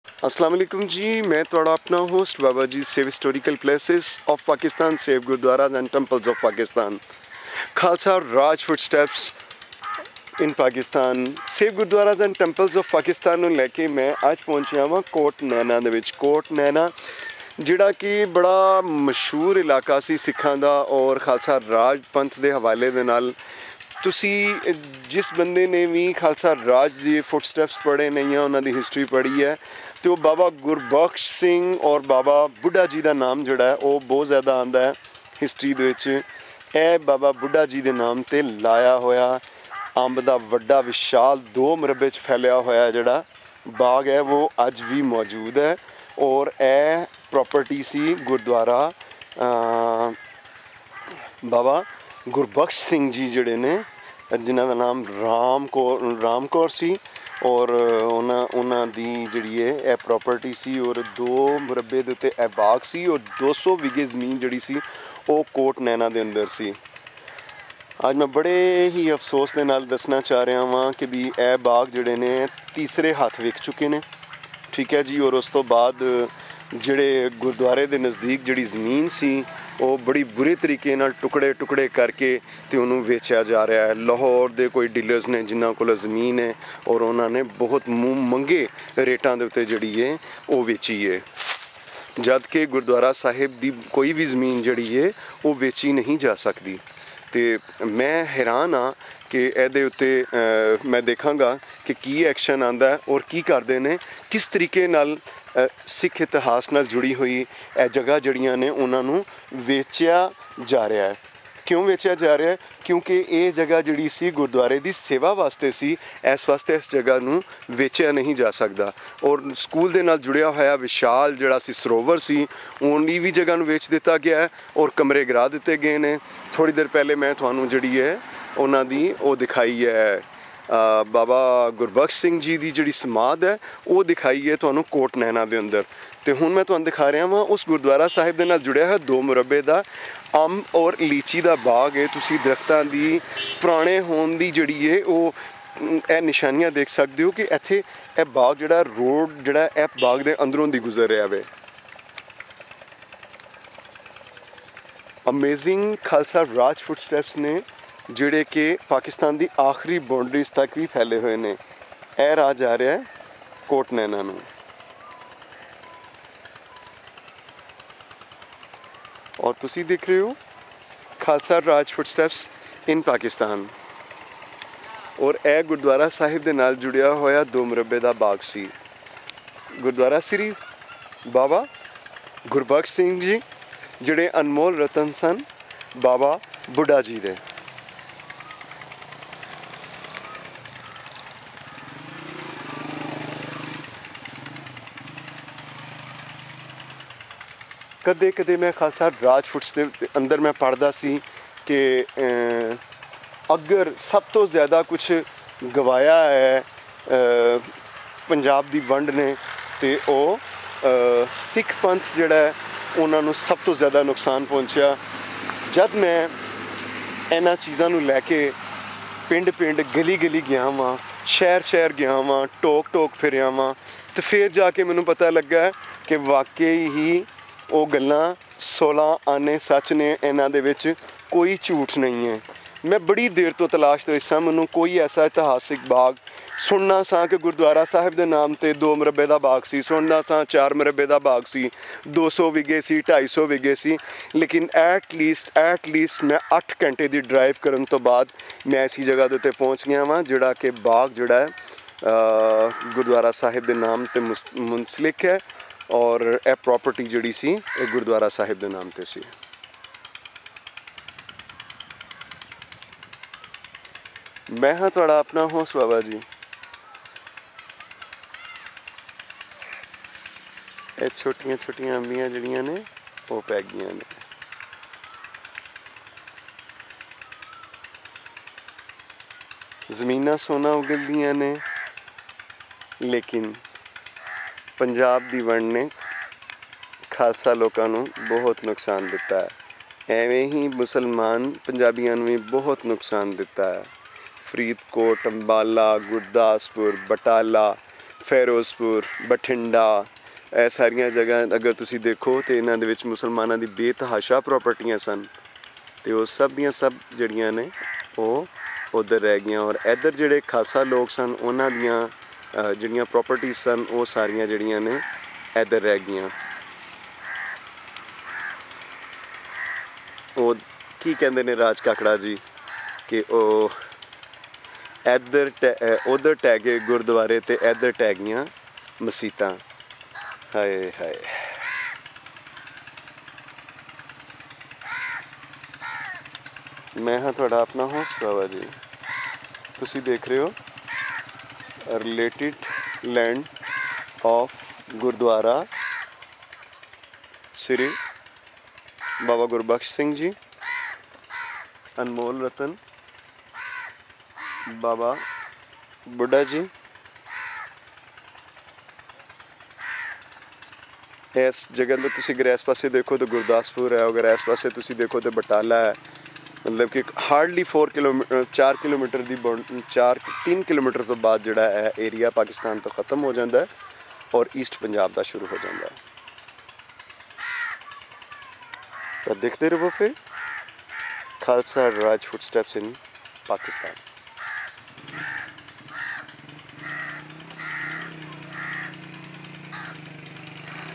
Mango Garden of Gurdwara Bhai Gurbaksh singh ji Kot Naina Shakargarh
Mango Garden of Gurdwara Bhai Gurbaksh singh Anmol Ratan of Baba Buddha Ji Kot Naina Tehsiel Shakar Garh District Naorwal Punjab Pakistan